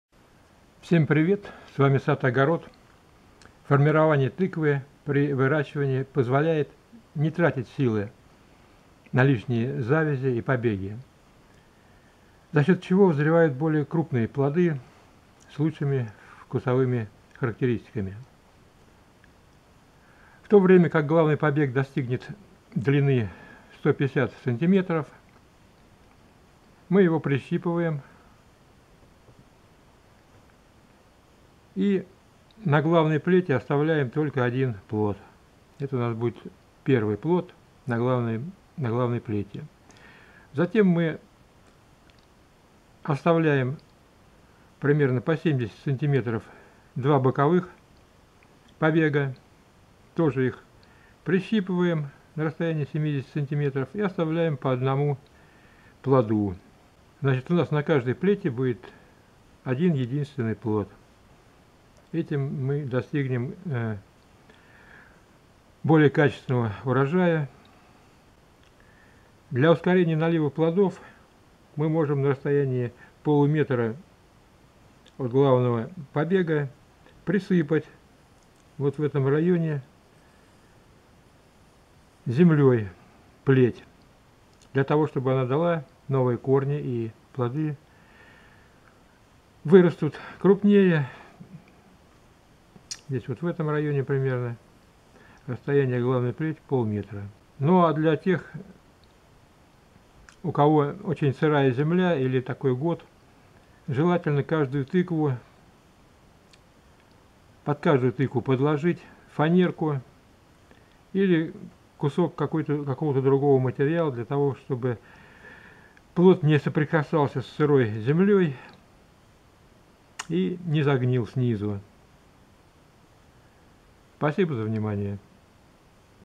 Схема формирования тыквы
Схема формирования тыквы. Схема формирования плетей тыквы в открытом грунте.   Желаю удачи!